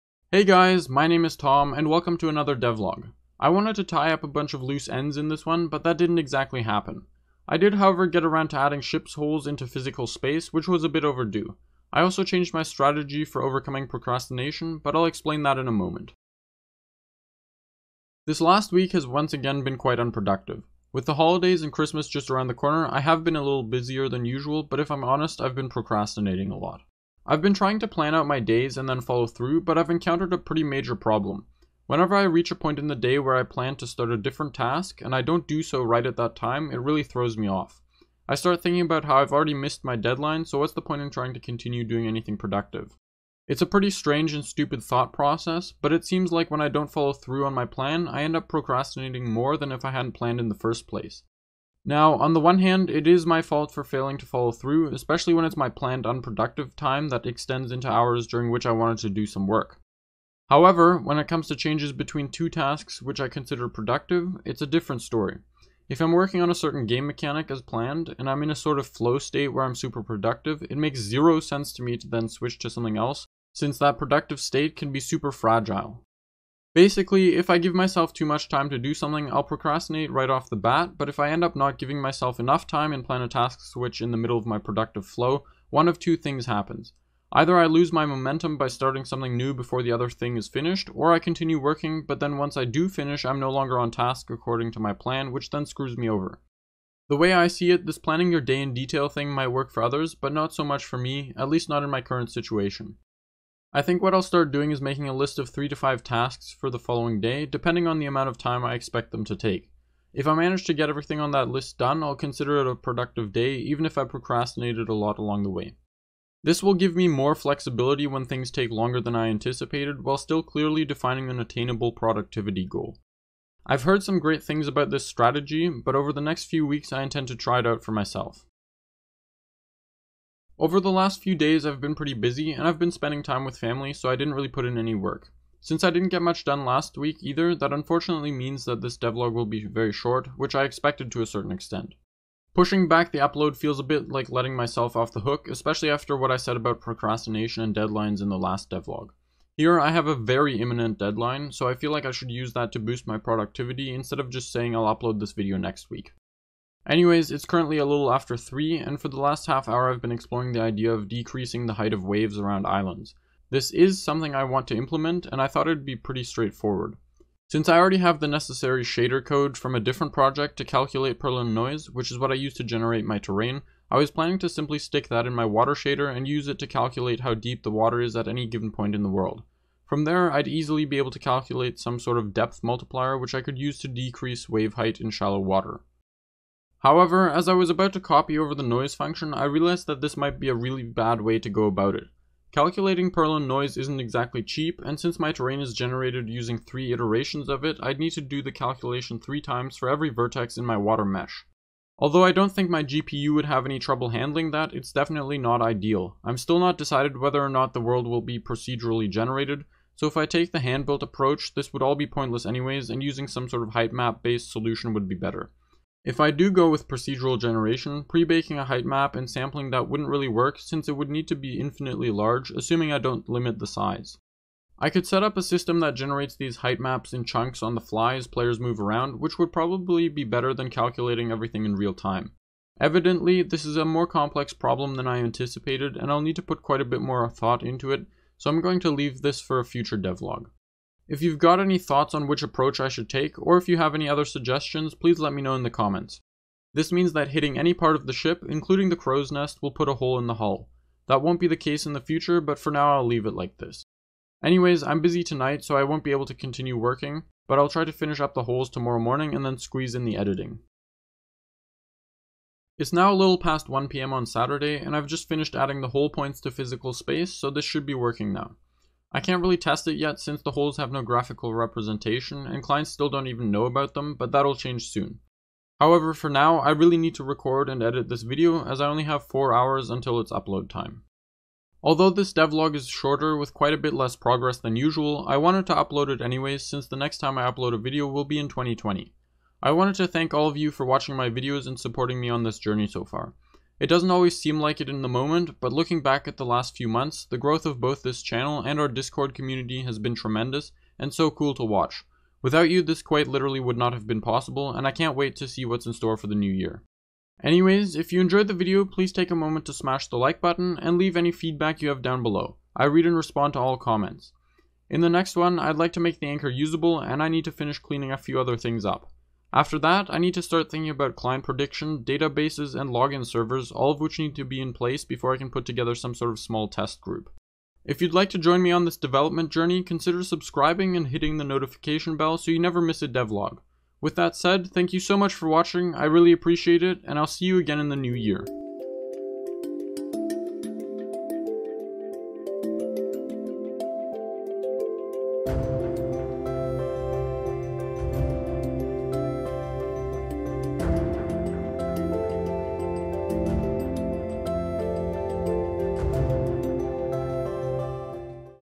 Physical Holes in Ships and Happy New Year! | Unity Multiplayer Game Devlog #22
In this Unity multiplayer game devlog, I finally add holes in ships to the physical world.

This devlog is a little shorter than usual, partially because I spent some time with family over Christmas, but also because I procrastinated a bunch. I've decided to try a different strategy to hopefully overcome that, since planning out my days wasn't exactly working for me. Regardless, I finally added holes in ships to the physical world, which was a bit overdue. I still need to make it so clients can see those holes and repair them, but all the server-side stuff is there now.